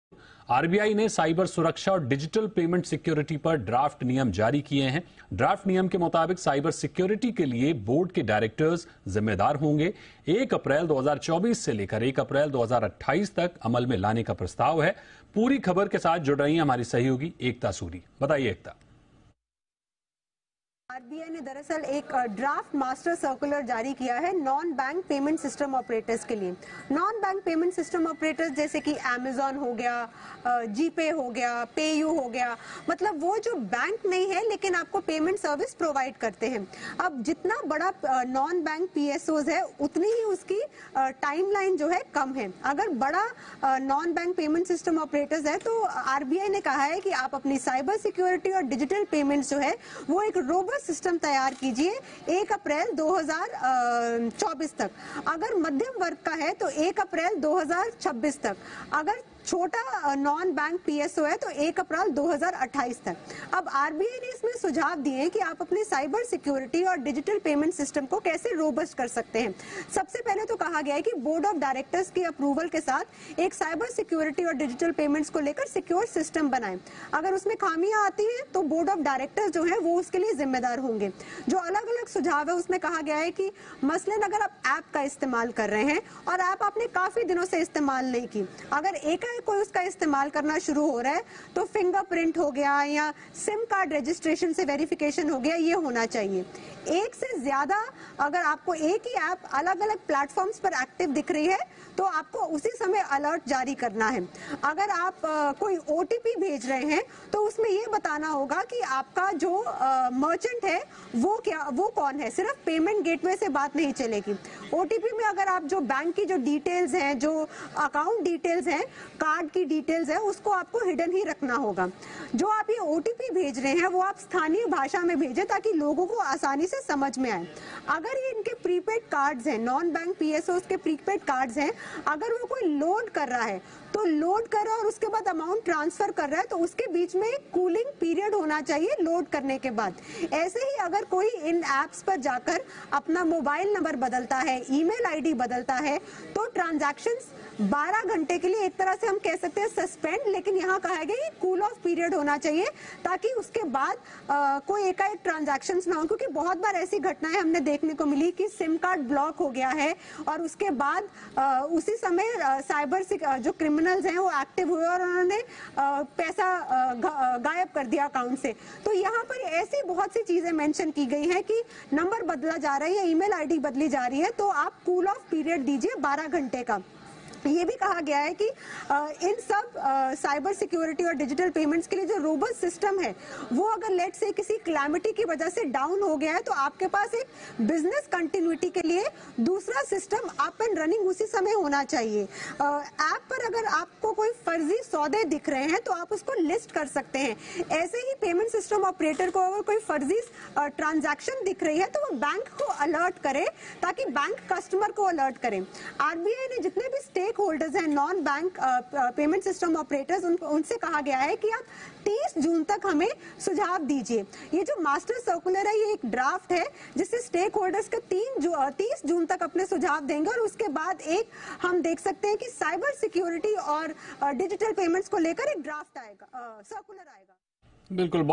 Tightening Cybersecurity: RBI Releases Draft Rules on Cybersecurity and Digital Payment Security
साइबर सुरक्षा के नियम हुए कड़े, RBI ने साइबर सुरक्षा, डिजिटल पेमेंट सिक्योरिटी पर ड्राफ्ट नियम जारी किया